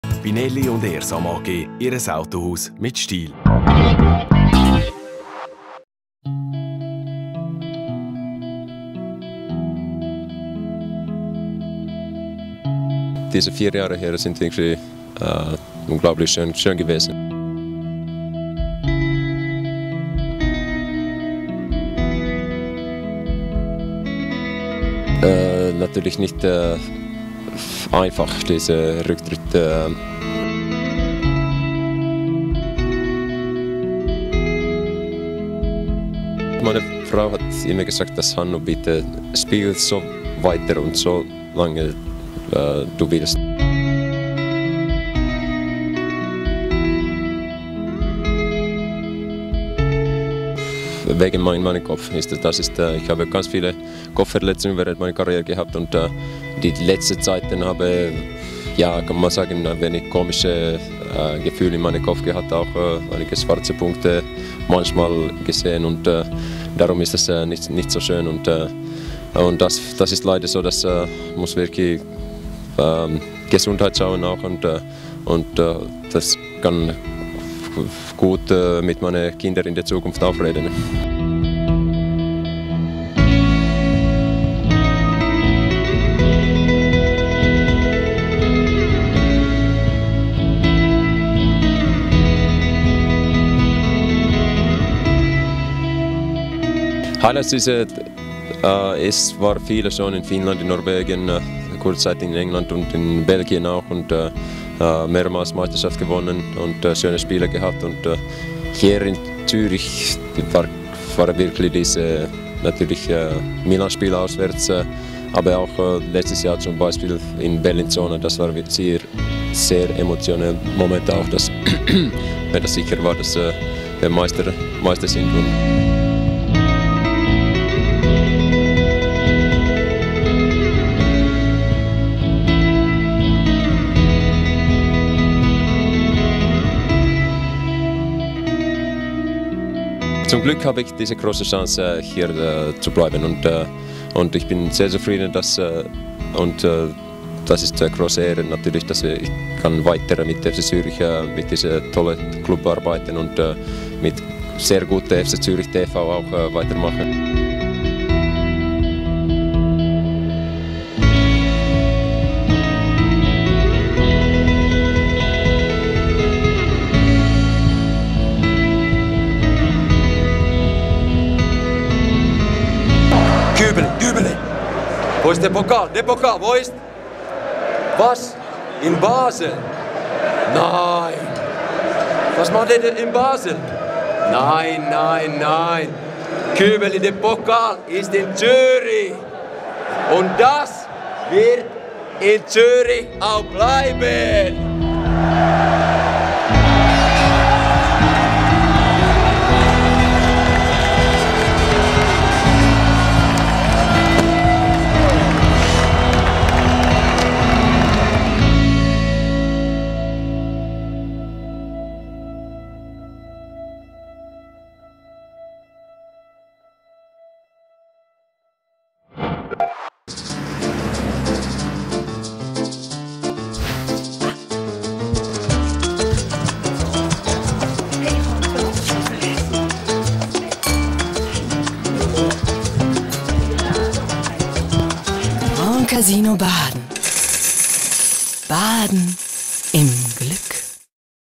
FCZTV CHANNEL: Danke Hannu
Hannu Tihinen beendet seine Spielerkarriere

Der bald 34-jährige Finne wird seinen im Sommer auslaufenden Profivertrag nicht verlängern. Mit dem letzten Saisonspiel am 16. Mai in der Neuenburger Maladière wird der FCZ-Captain seine wunderbare Karriere beenden. Hannu Tihinen wechselte im Sommer 2006 vom RSC Anderlecht zum FC Zürich. Zuvor spielte er für West Ham United, Viking Stavanger, HJK Helsinki und KePS Kemi. Tihinen bestritt 77 Spiele für die finnische Nationalmannschaft, dabei erzielte er 5 Tore. Sein wohl berühmtestes Tor erzielte er für den FC Zürich und zwar am 30. September 2009 beim 1:0-Sieg des FCZ in der UEFA Champions League im San Siro-Stadion gegen die AC Milan.

Wir freuen uns mitteilen zu dürfen, dass Hannu Tihinen weiterhin für den FC Zürich tätig sein wird und zwar als Direktionsassistent. Zu seinen neuen Hauptaufgaben gehören unter anderem:

- Neue Spieler betreuen und in die 1. Mannschaft integrieren

- Betreuer und 1. Ansprechsperson für an andere Vereine ausgeliehene Spieler

- Berater der FCZ-Nachwuchsspieler betreffend Karrierenplanung

- Zusammen mit Massimo Rizzo Verantwortlicher für individuelles Verteidigertraining beim FCZ-Nachwuchs

Hannu Tihinen wird zudem eine Trainerausbildung beginnen.
Die Rückennummer 30 wird künftig zu Ehren des Publikumslieblings beim FCZ vorerst nicht mehr vergeben.

Der FC Zürich dankt Hannu Tihinen für sein stets vorbildliches Engagement und wünscht ihm in seiner neuen Funktion viel Erfolg.